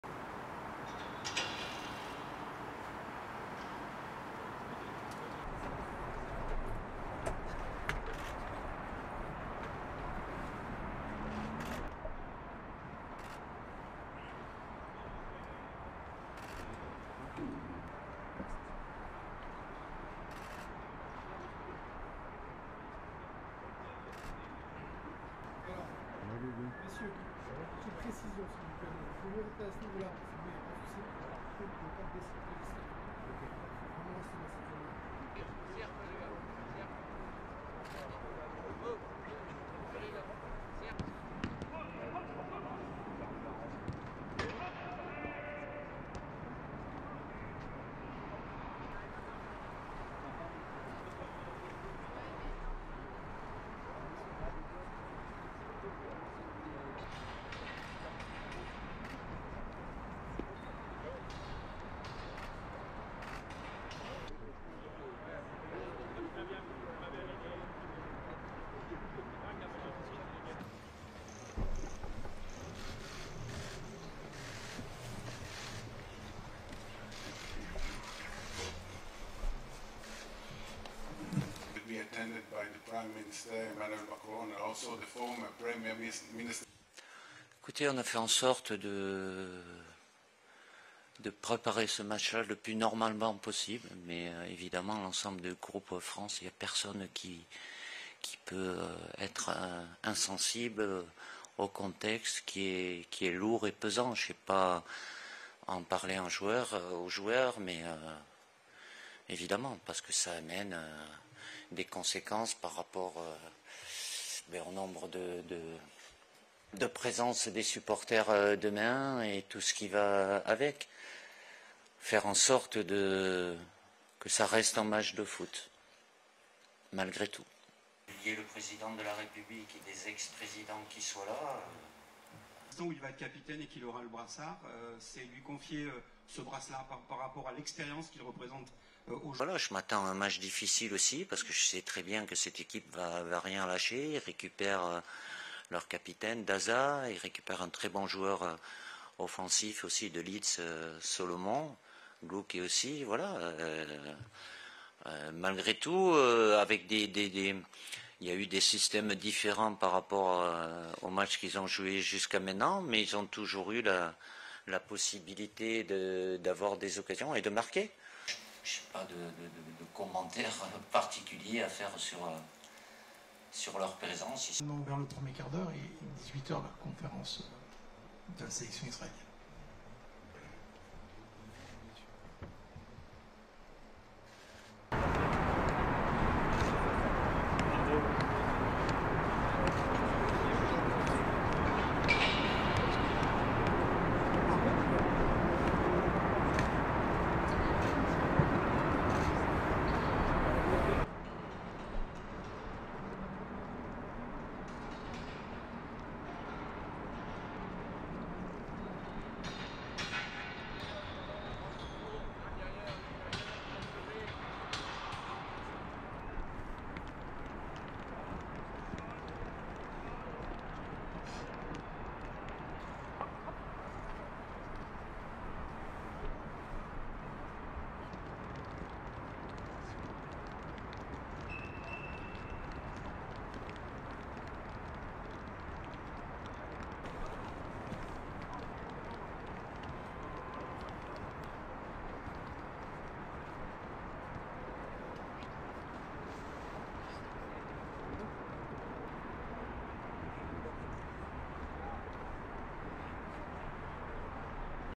Heavy atmosphere around game vs Israel felt by French team, says coach
France's football coach Didier Deschamps said on Wednesday (November 13) his team was not immune to the 'heavy' context of its upcoming match with Israel, following violent clashes between locals and Israeli soccer fans in Amsterdam last week.

Authorities have stepped up security in Paris ahead of a France-Israel soccer match on Thursday (November 14), hoping to avoid a repeat of the clashes in Amsterdam.

The Nations League match at the Stade de France comes at a fraught moment, with diplomatic relations between French President Emmanuel Macron and Israeli leader Benjamin Netanyahu strained by Israel's war in Gaza.

Some 4,000 police will secure the event, deployed in the stadium, outside the ground and on public transport, the Paris police force said.

Only French and Israel flags would be allowed inside the stadium, he added.

Macron will attend the game in a show of solidarity.

Still, turnout will likely be low, with just 20,000 fans expected in the 80,000 capacity stadium north of Paris.

"We'll make sure that it remains a football match despite everything," Deschamps said.

He also said he expects a difficult match, with the presence of Israel's key players, captain Eli Dasa and midfielders Manor Solomon and Oscar Gloukh.

Star forward Kylian Mbappe is left out of France's squad, but Deschamps did not specify if it was due to injury.

(Production: Julien Pretot, Lucien Libert, Michaela Cabrera)